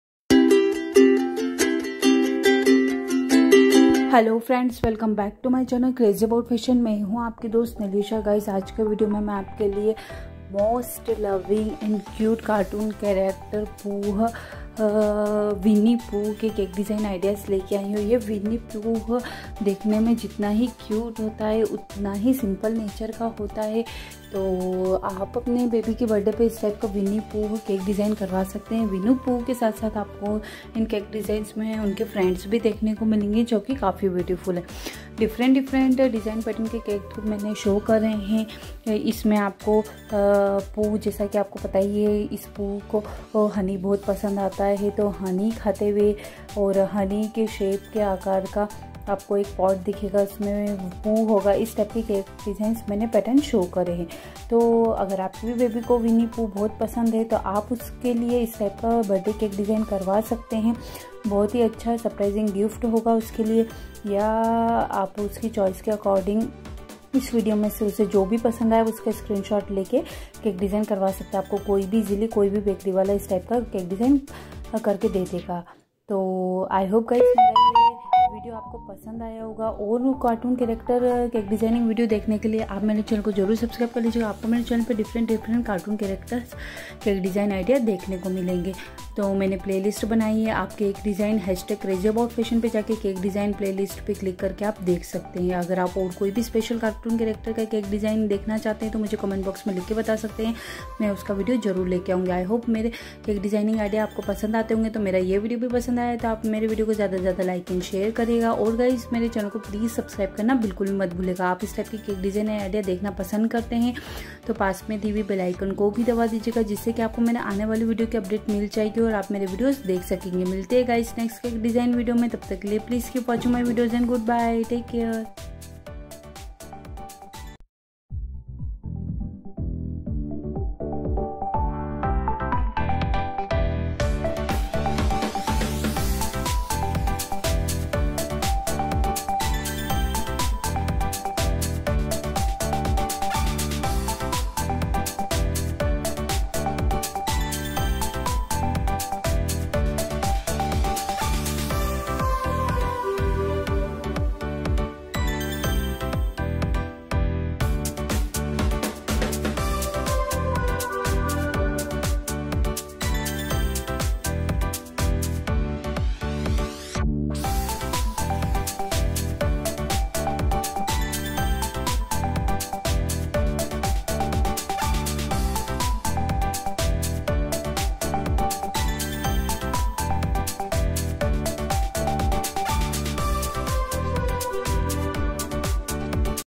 Very cute Winnie Pooh cake design | Pooh and his friends cake ideas - Crazy about Fashion.
Winnie pooh cake design .
In thisWinner pooh cake design .
In this video we show you kids favourite cartoon character Pooh cake design for birthday.All cake design  are very beautiful and attractive.Your baby really like it..